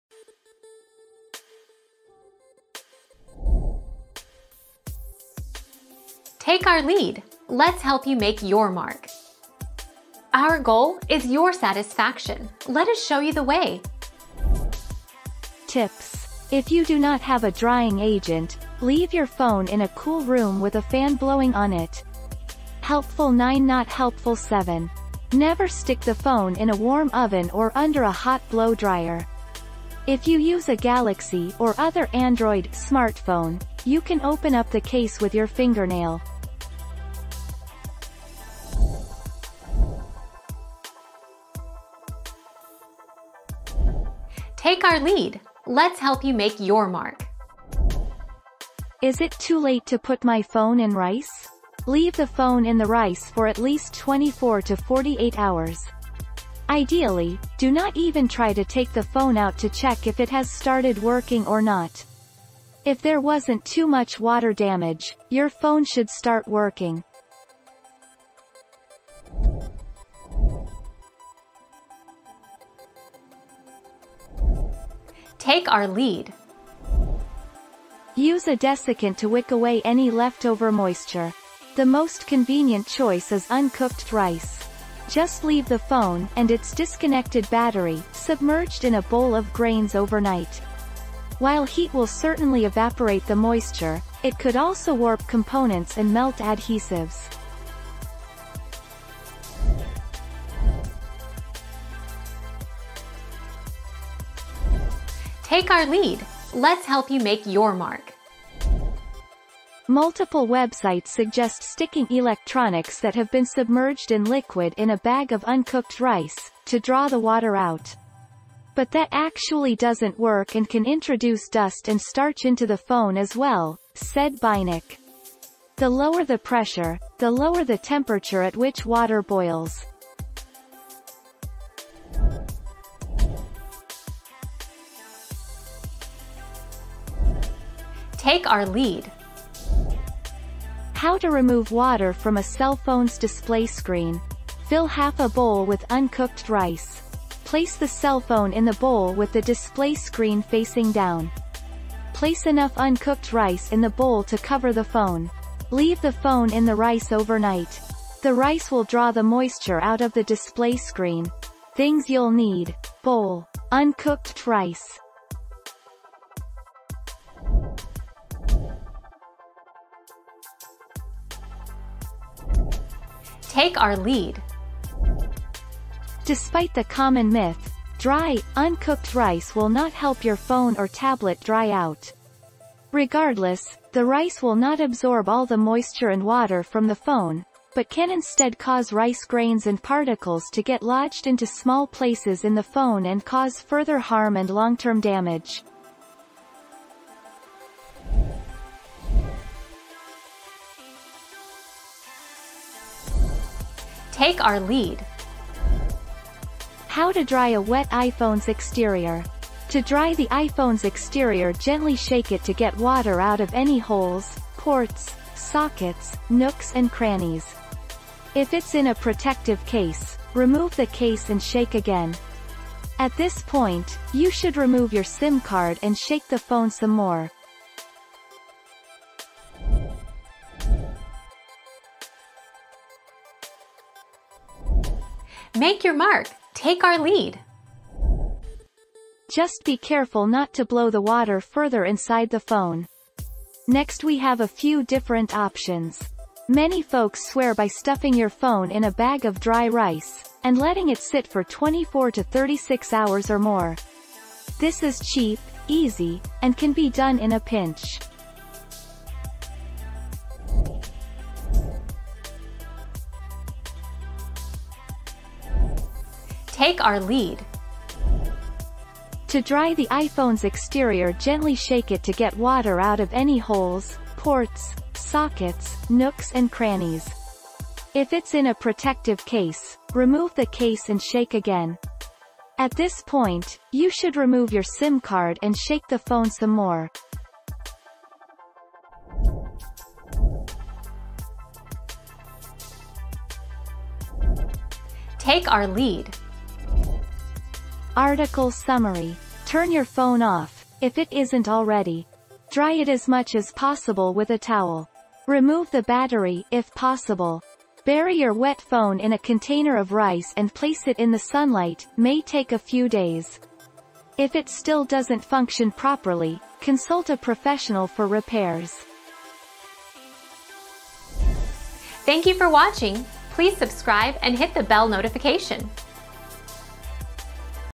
How do you get water out of your phone without rice?
00:00 - How do you get water out of your phone without rice?
00:40 - Is it too late to put my phone in rice?
01:11 - Does Rice help a wet phone?
01:41 - Should I put my phone in rice?
02:13 - How do you get moisture out of your phone screen?
02:48 - Is rice bad for phones?
03:19 - How do you get water out of an iPhone without rice?
03:50 - How do you dry out a phone that fell in water?
04:21 - How can I dry my iPhone without rice?
04:49 - How do I get moisture out of my phone?

Laura S. Harris (2021, January 21.) How do you get water out of your phone without rice?
    AskAbout.video/articles/How-do-you-get-water-out-of-your-phone-without-rice-211910